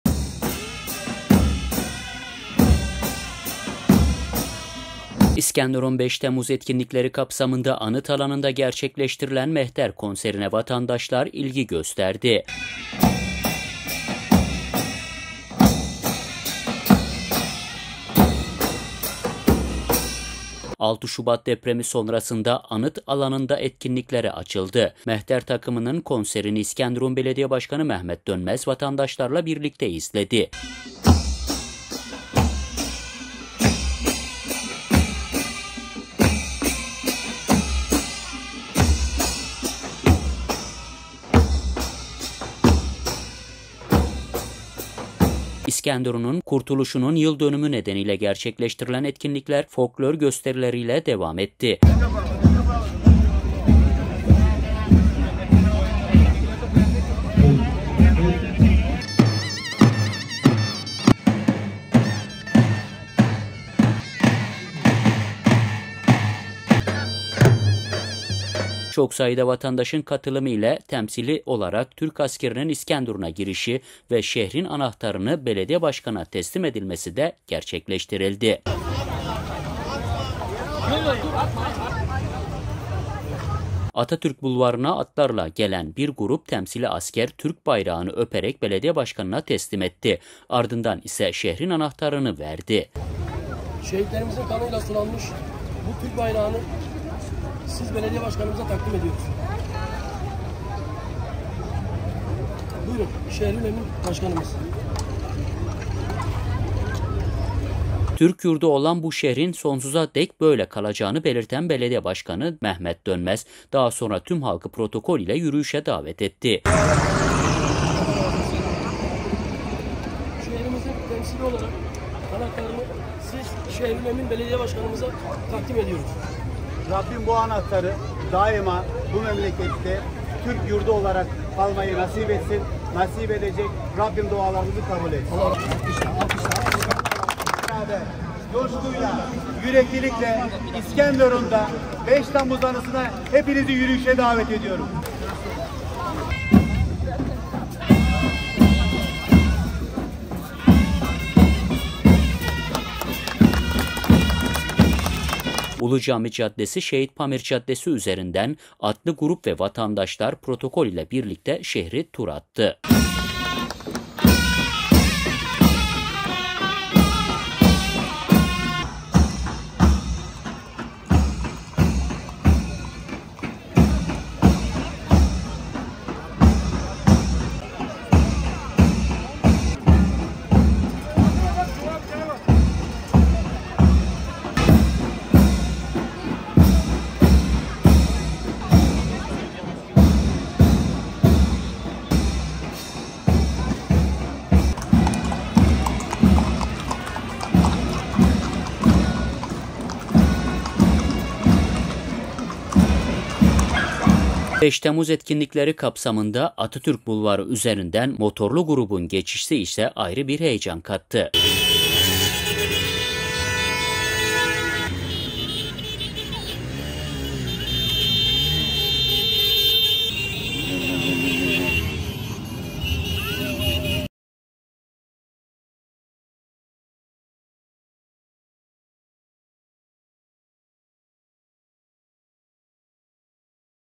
5 TEMMUZ KÜLTÜR VE SANAT FESTİVALİNDE MEHTER TAKIMI KONSERİ İLGİ GÖRDÜ
İskenderun 5 Temmuz etkinlikleri kapsamında Anıt alanında gerçekleştirilen Mehter konserine vatandaşlar ilgi gösterdi.